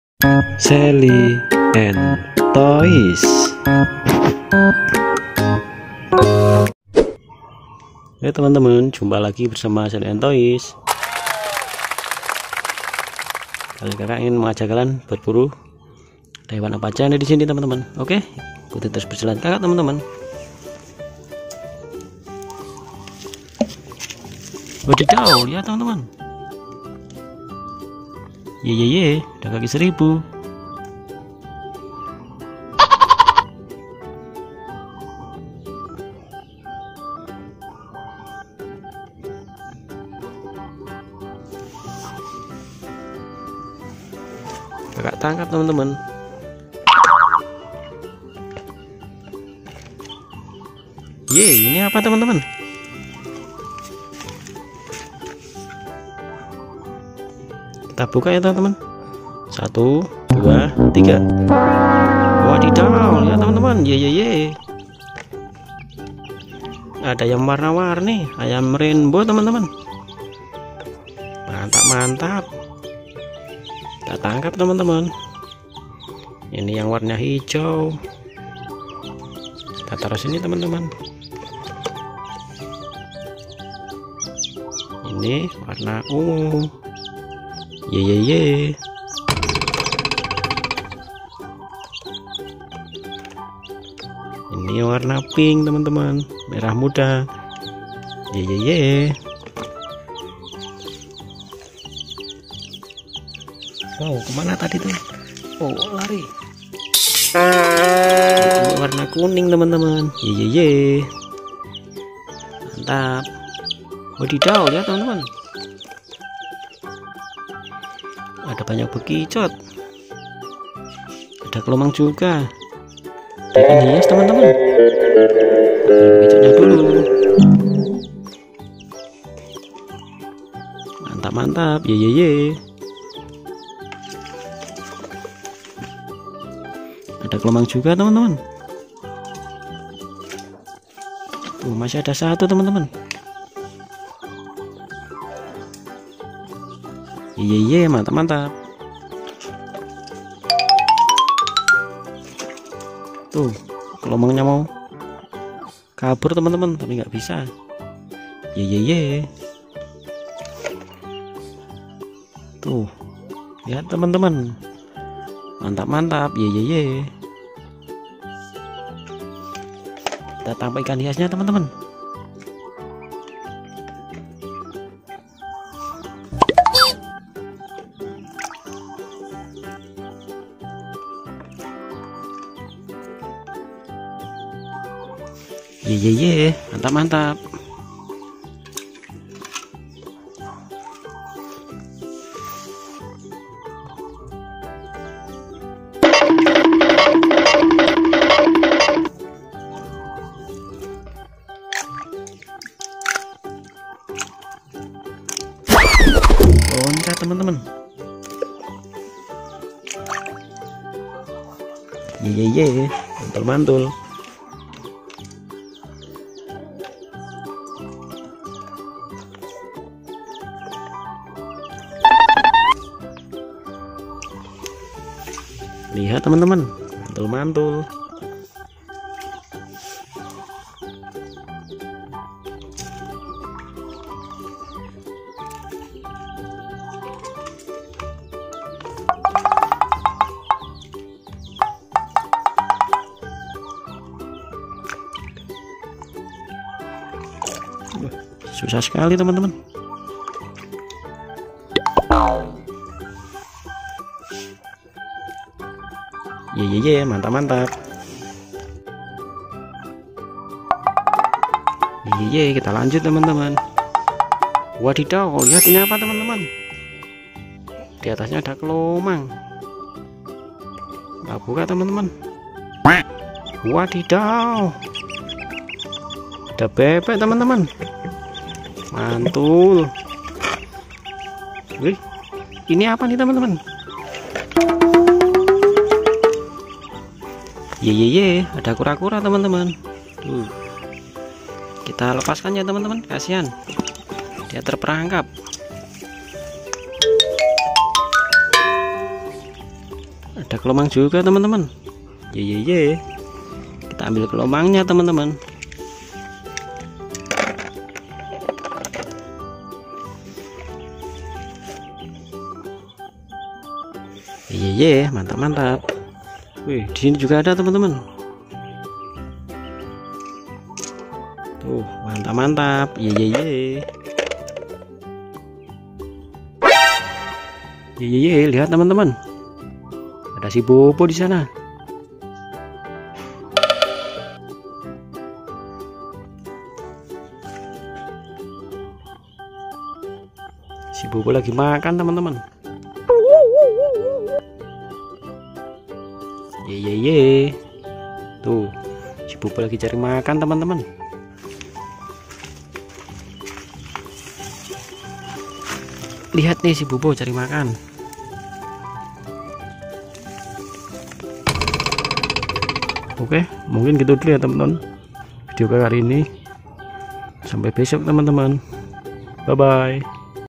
Asyik temukan ayam warna warni,kura kura,kaki 1000, bekicot, kelomang,bebek.
hai teman teman semua jumpa lagi bersama @seliandtoys2273
kali ini kakak ingin mengajak teman teman semua untuk berpetualang bersama kita akan mencari hewan yang unik dan menarik yang ada di sekitar kebun rumah kita.
sahabat @seliandtoys2273 kita bersama sama belajar mengenal hewan yang ada di sekitar kita ya teman teman bagaimana kehidupan mereka sehari-hari.
seli and toys
ikan hias
kaki seribu
ayam warna warni